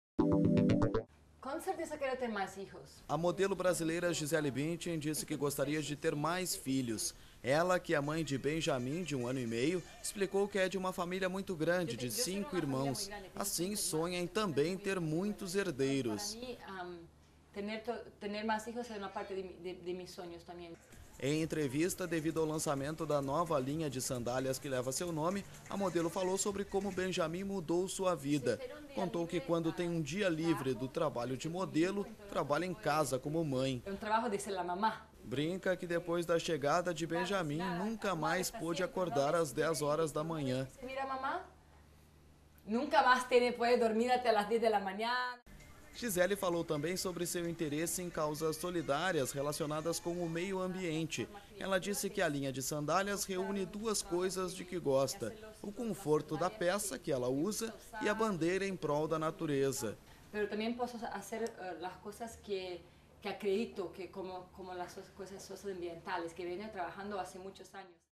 Gisele Bündchen diz que gostaria de ter mais filhos
Madri (Espanha), 7 abr (EFE).- A modelo brasileira Gisele Bündchen gostaria de ter mais filhos, mas, por enquanto, a única coisa que pede ao futuro é "saúde".
"Só espero que a vida me dê saúde, porque isso é o que posso esperar. É impossível prever o futuro. Quero estar com saúde e rodeada das pessoas que eu amo e que são importantes para mim, como minha família e meus amigos", diz. Em entrevista, devido ao lançamento da nova linha de sandálias que leva seu nome, a modelo falou de seu filho Benjamin e da maneira que ele mudou sua vida. Gisele falou também sobre seu interesse em causas solidárias relacionadas com o meio ambiente.